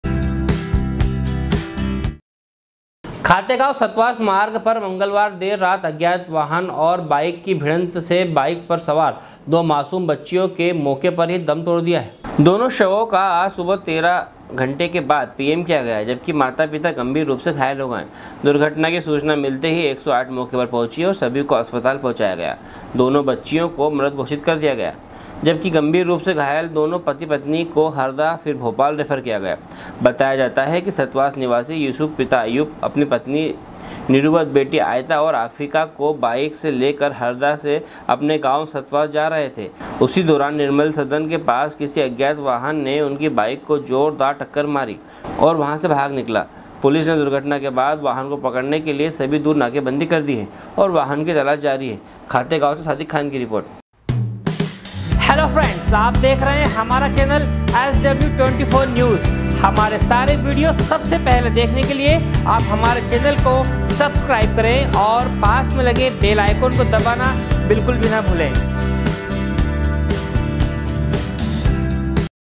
SW 24 News Madhyapradesh#khategaon#28.05.2020
sw sw24news sw24newsmadhyapradesh livenews news24खातेगांव सतवास मार्ग पर मंगलवार देर रात अज्ञात वाहन ओर बाइक की भिड़ंत में बाइक पर सवार दो मासूम बच्चियो ने मोके पर ही दम तोड दिया।
खातेगांव से सादिक खान की रिर्पोट।